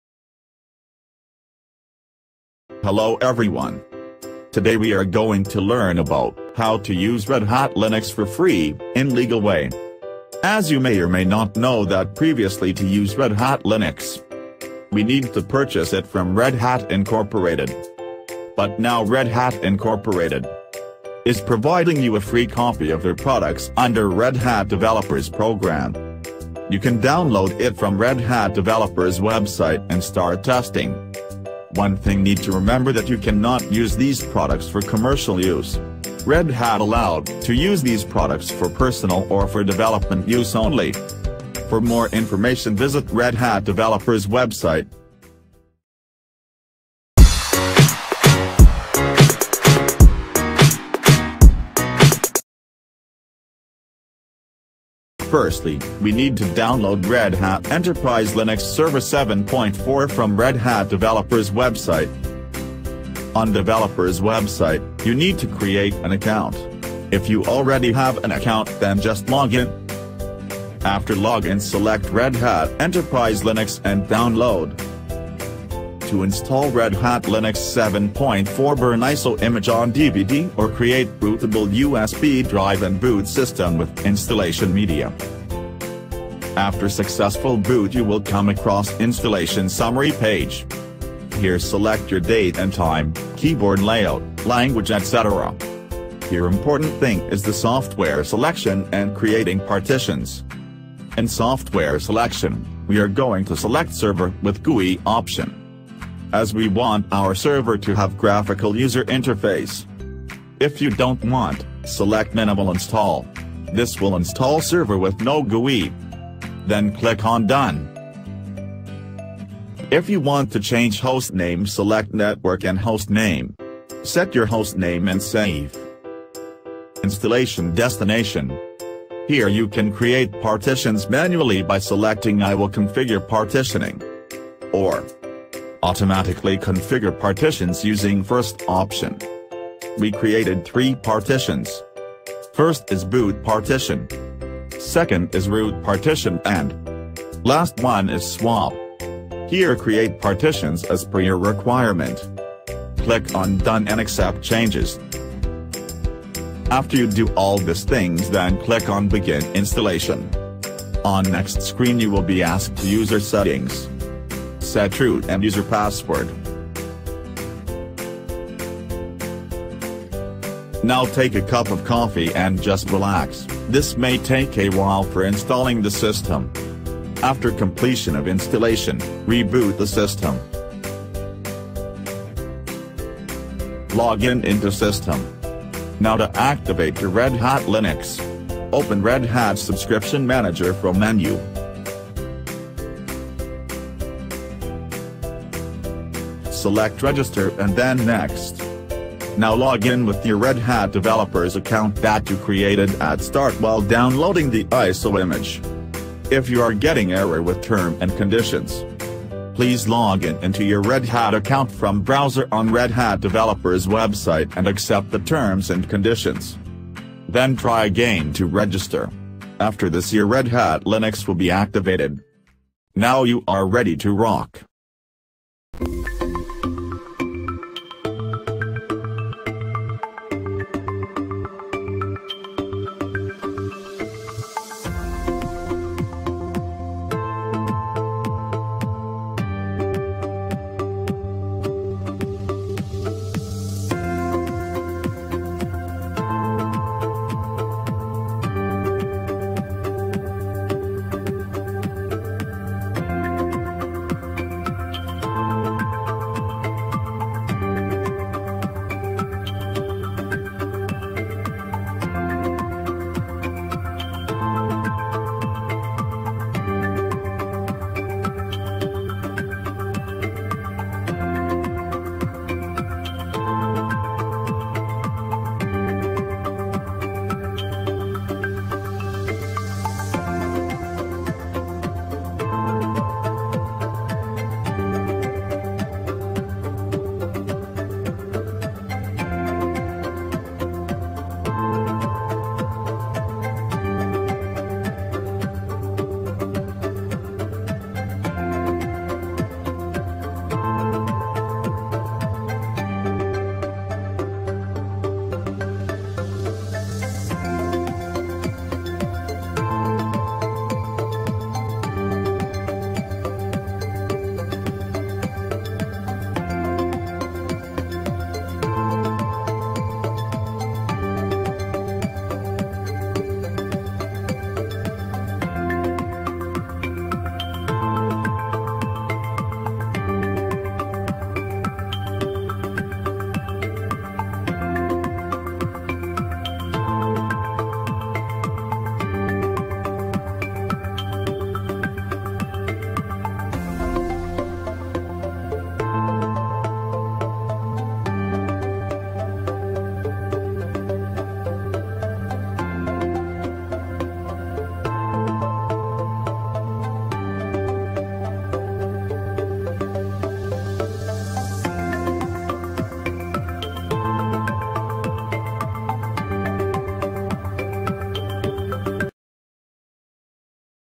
[Legal] RedHat Enterprise Linux Server 7 4 Installation and Activating RedHat subscription for free
In this tutorial we are going to learn, How to install Redhat Enterprise Linux Server 7.4 and how to activate Redhat subscription for free. Under Redhat Developers Program.